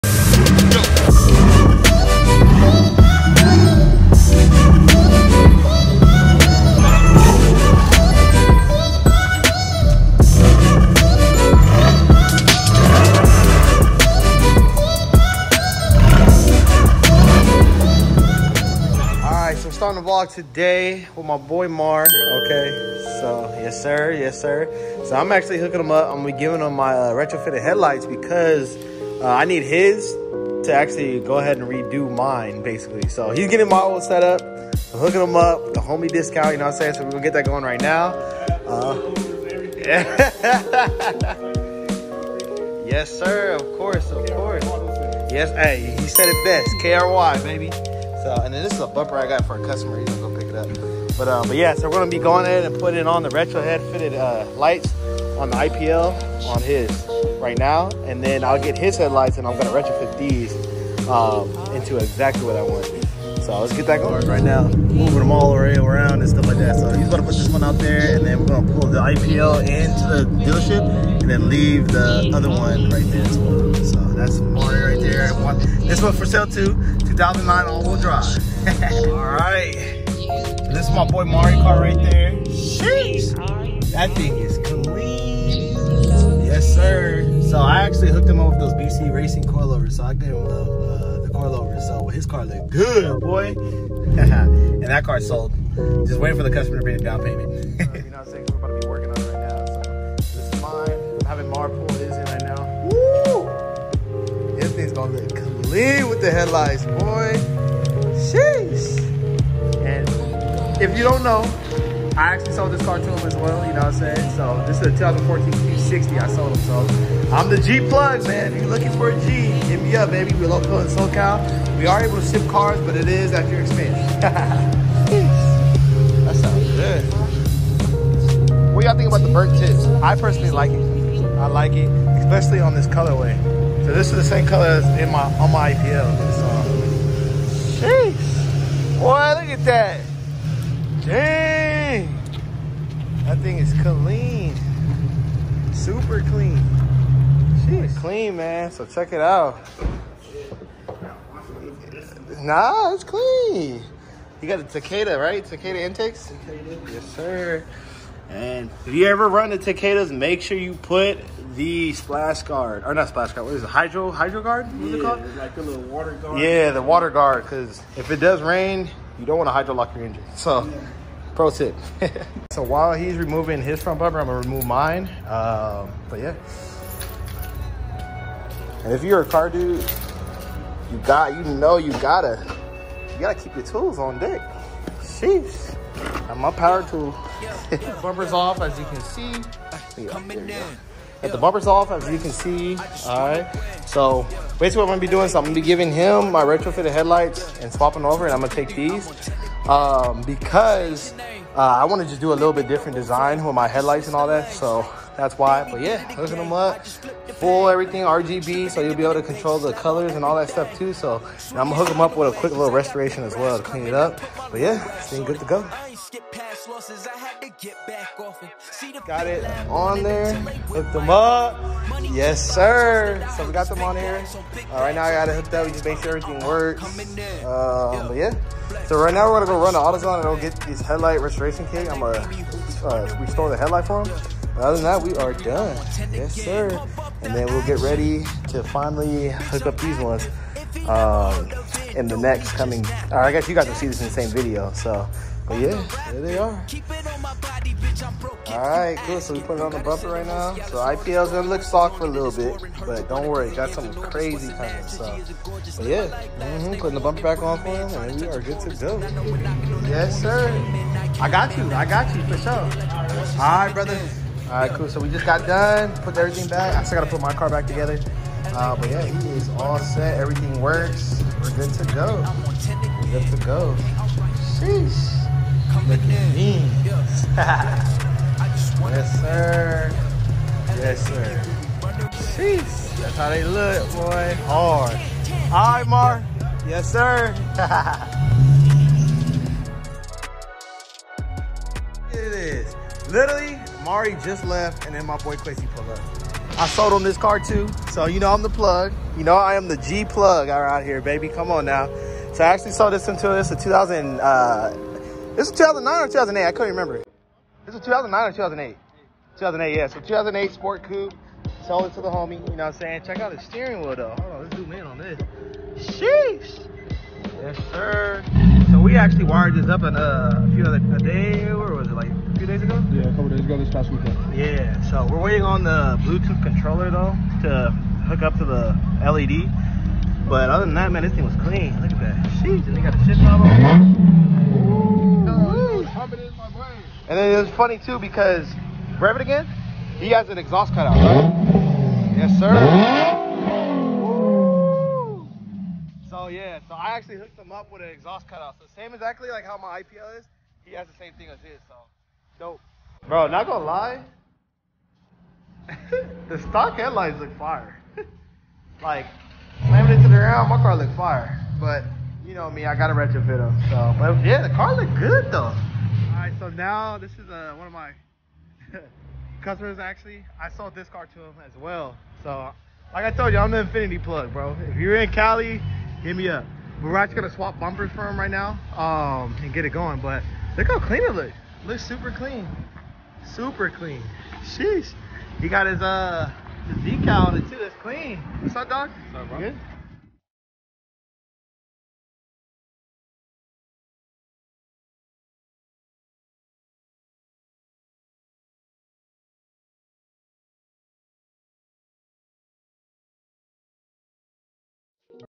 A DAY IN THE LIFE OF A INFINITI/G37 DEALER!!!
A quick Vlog of me at my Small Dealership!!